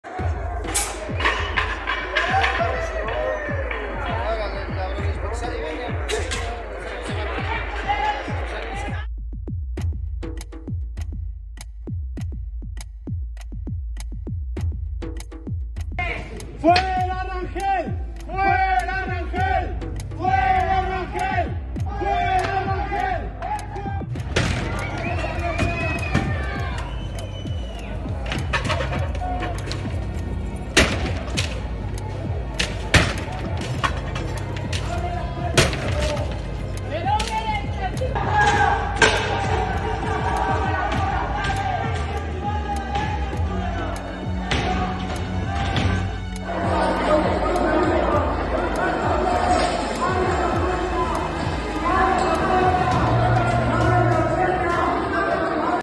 Comunidad LGBTTTI+ desata su coraje contra el Congreso CDMX; lo apedrean y pintan
Integrantes de colectivos ´trans´ manifestaron su molestia contra la iniciativa de la panista América Rangel.